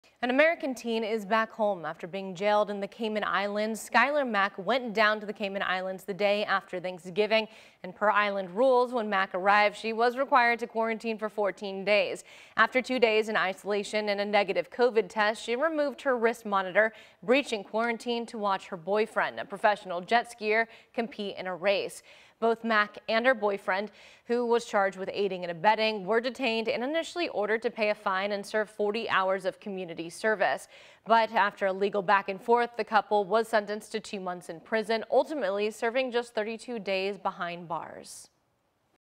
American teen who violated Cayman Islands COVID-19 rules released from jail
Skylar Mack, 18, of Georgia, was sentenced to two months after pleading guilty to violating Cayman Islands quarantine rules. She served only 32 days behind bars.